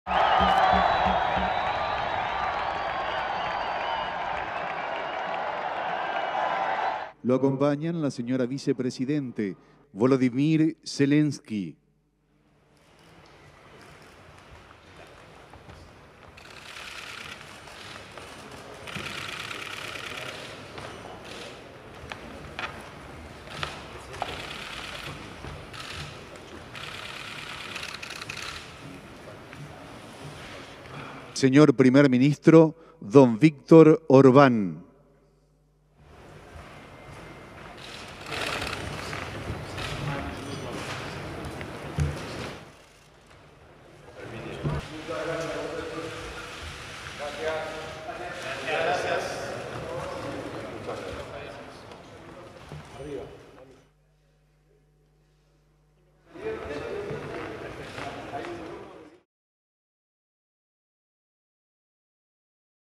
Argentina's President Milei welcomes world leaders after swearing in
Javier Milei, Argentina’s new president, welcomed world leaders to Casa Rosada, the government house. Among them were President Zelensky of Ukraine, Viktor Orbán, prime minister of Hungary, and President Boric of Chile. President Milei, a right-wing libertarian, was sworn in as president on Sunday.

Argentina’s new president took office with a pledge to use “shock treatment” to revive the economy, but warned that the country’s financial crisis would get worse in the short term.

In his first speech since taking office on Sunday, President Milei pledged sharp spending cuts as he blamed the previous government for rampant inflation, which has risen above 140 per cent and is expected to hit 200 per cent by the end of the year.

“There is no alternative to shock treatment,” Milei told a crowd of supporters from the steps of congress. “There is no money.”